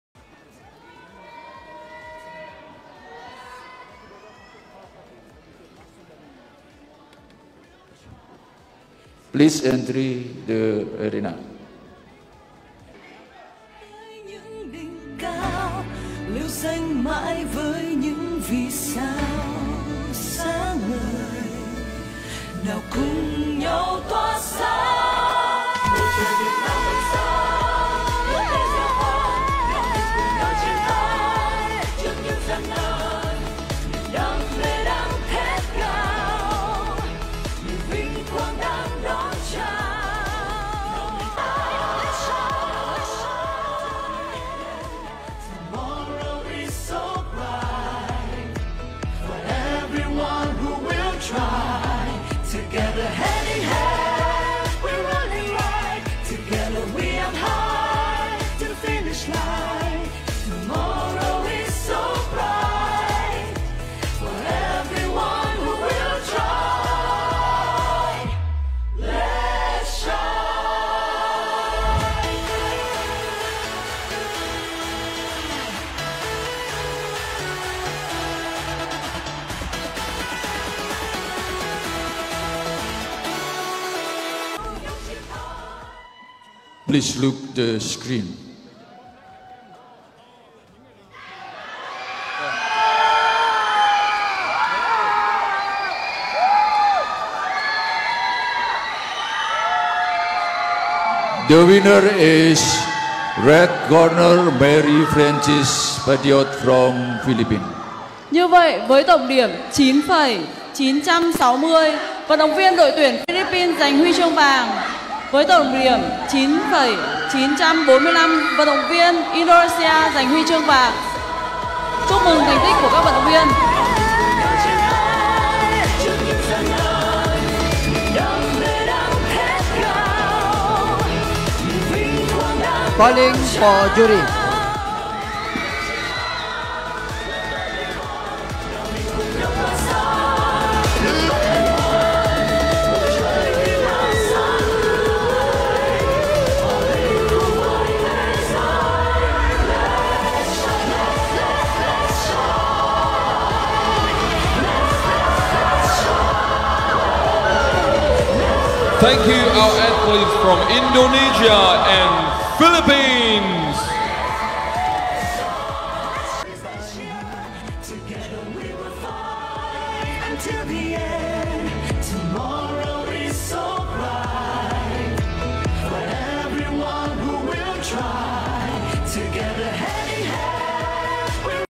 Philippine pride gold medalist PENCAK SILAT 2022
MARY FRANCINE PADIOS HIGHLIGHTS OF GOLD MEDAL PERFORMANCE AT THE 31st SEA GAMES IN VIETNAM

🔸PENCAK SILAT
▪️Women's Seni Tunggal or Artistic Single
FINALS: 🇵🇭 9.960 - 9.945 🇮🇩 Puspa Arum